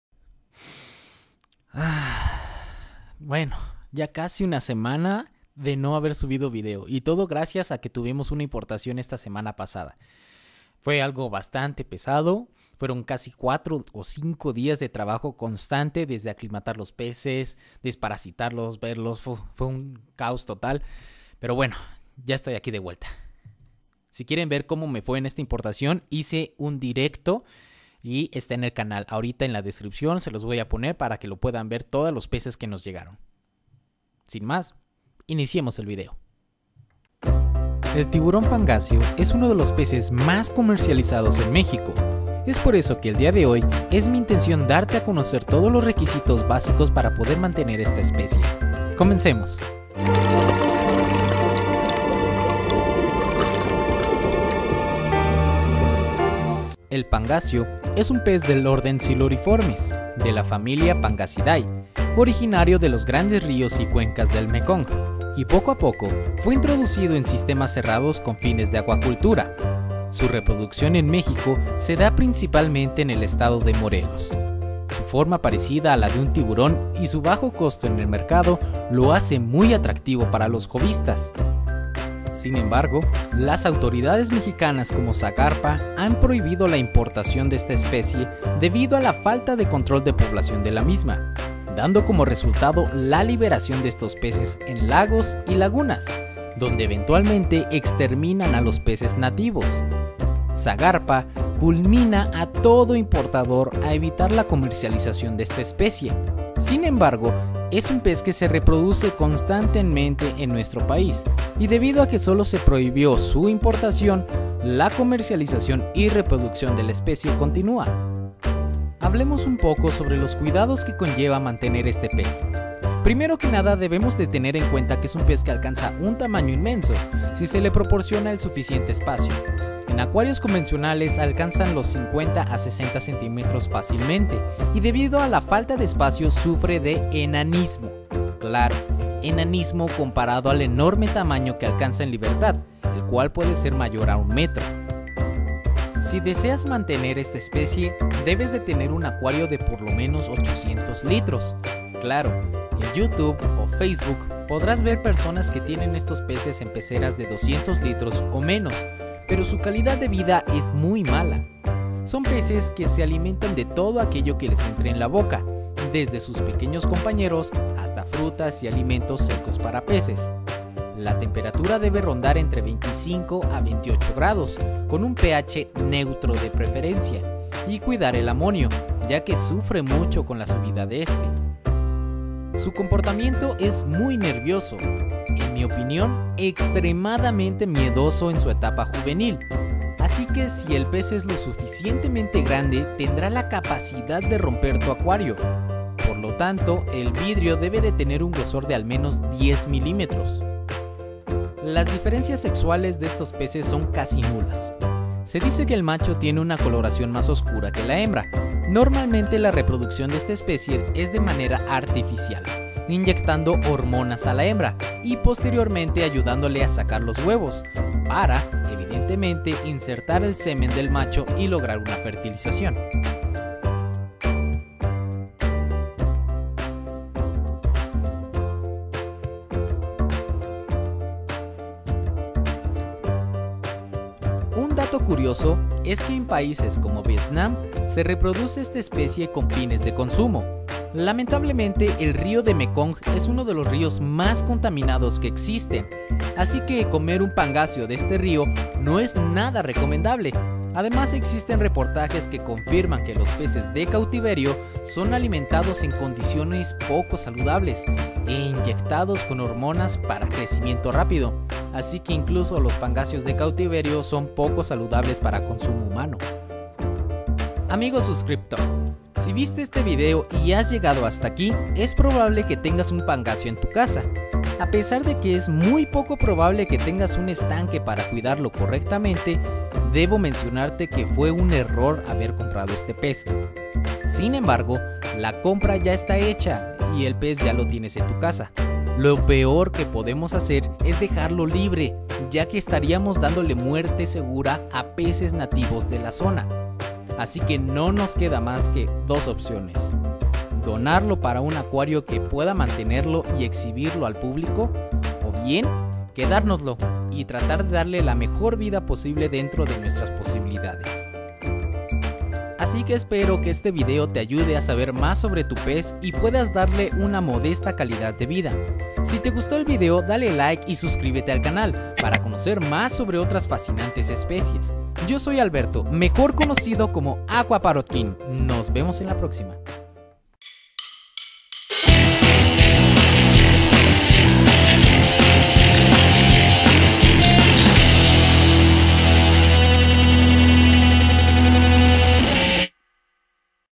Caracteristicas y Cuidados del Pangasio
Conoce todo lo que necesitas saber para cuidar a un tiburon pangasio.